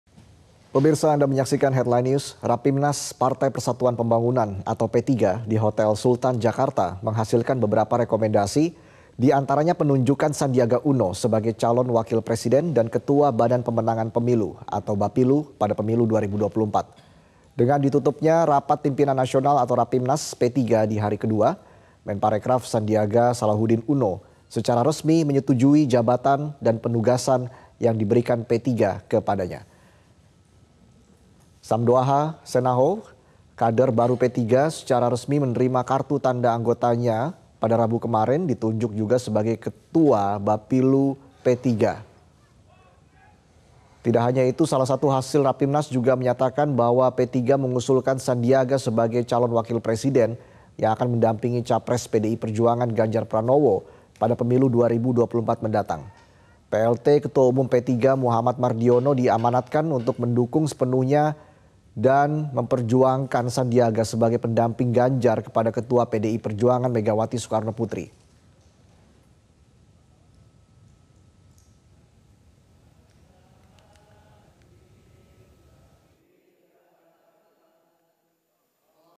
PPP Akan Usul Ke PDIP Agar Sandiaga Uno Jadi Cawapres
MetroTV, Partai Persatuan Pembangunan PPP Di Hotel Sultan Jakarta menghasilkan beberapa rekomendasi diantaranya ditunjukkan Sandiaga Uno sebgai Calon Wakil Presiden dan Badan Ketua Kemenangan Pemilu.